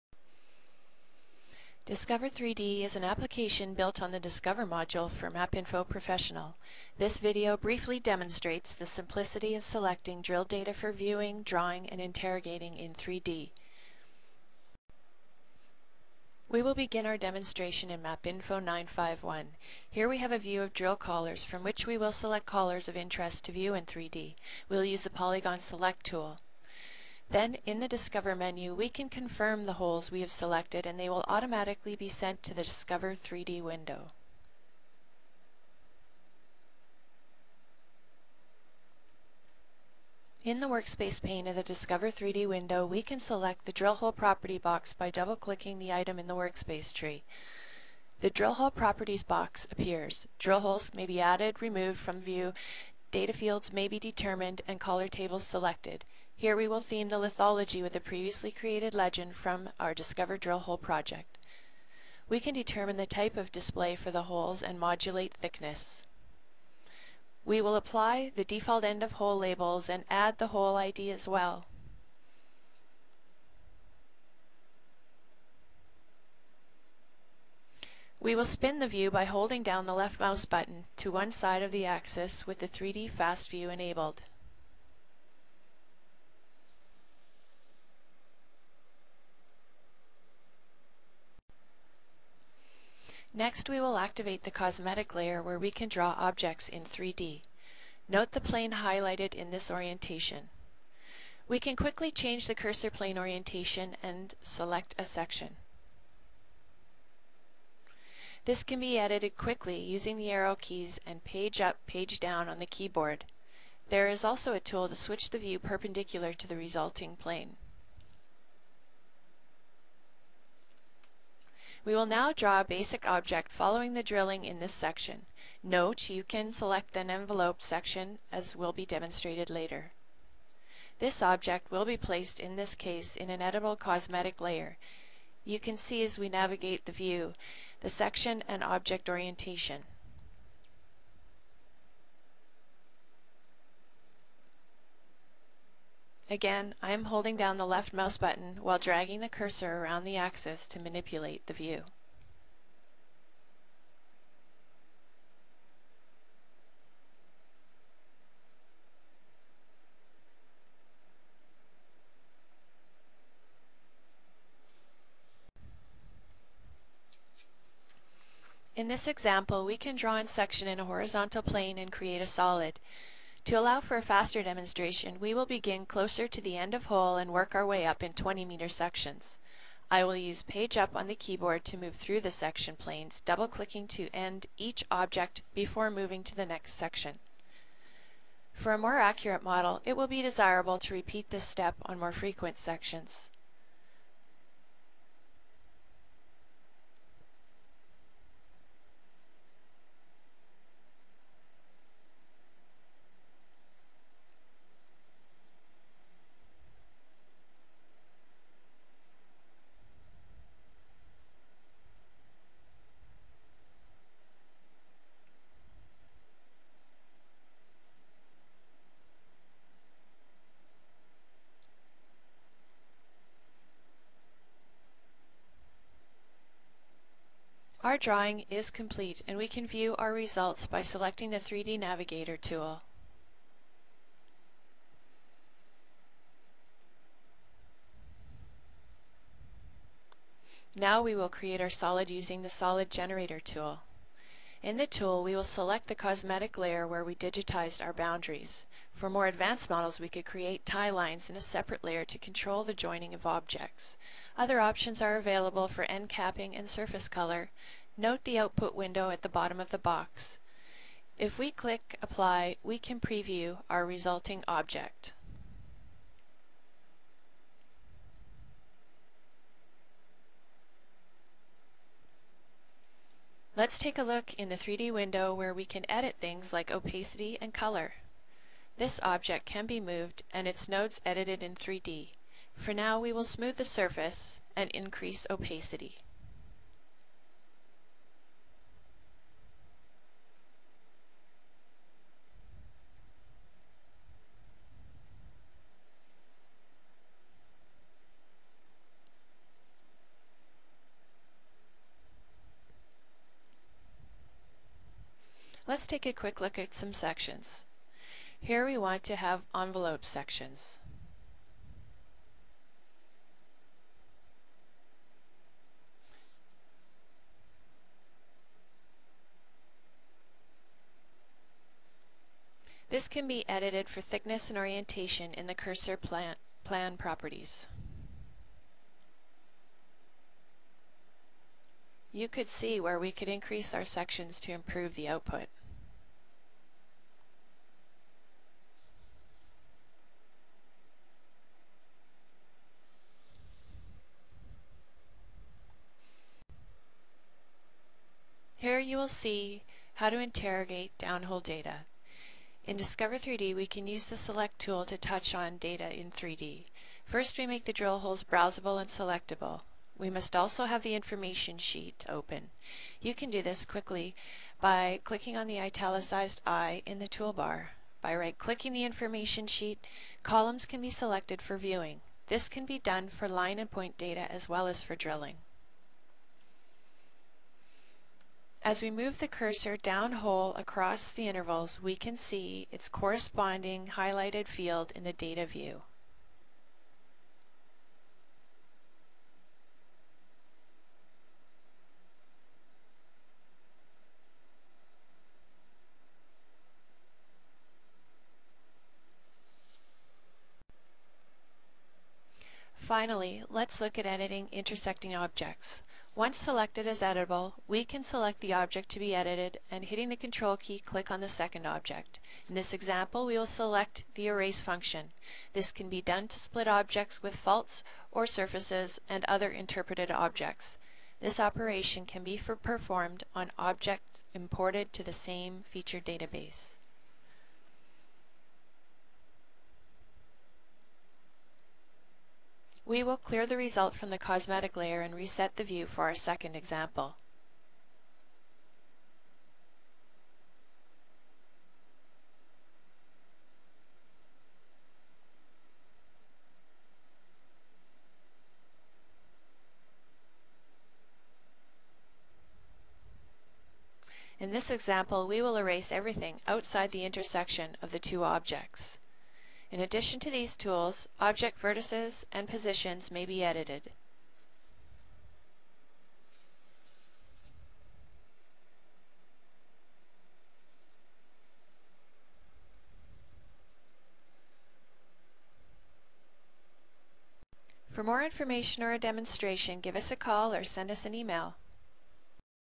Drilling Data, From 2D to 3D - Tetrad MapInfo and Discover Series
Use MapInfo 9.5, Discover 10.0 and Discover 3D 4.0 to view and interrogate drilling data in a 3D environment.